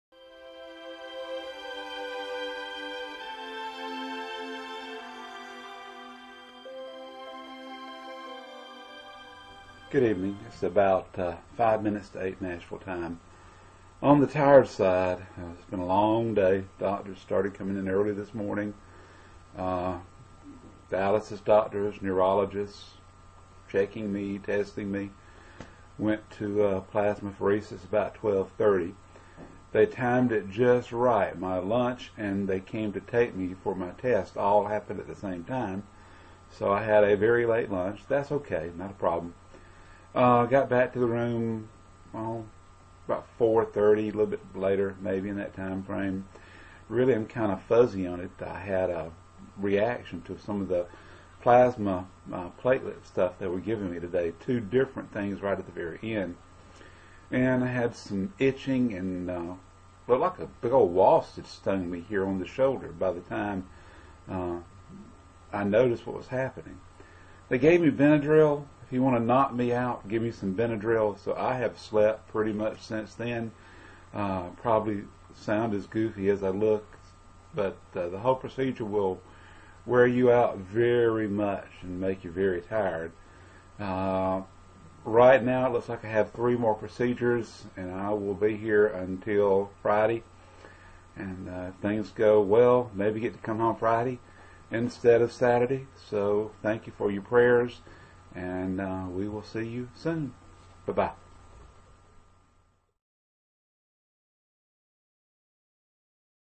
2010-03-09 Plasma Exchange Report 2 from Vanderbilt
In May 2009, I developed a series a severe neurological problems including: small fiber neuropathy, sensory-motor axonal neuropathy, autonomic neuropathy, and Myasthenia Gravis. These video chronicle my journey.  My book on this subject is Body Betrayed: My Journey through Sickness and Faith.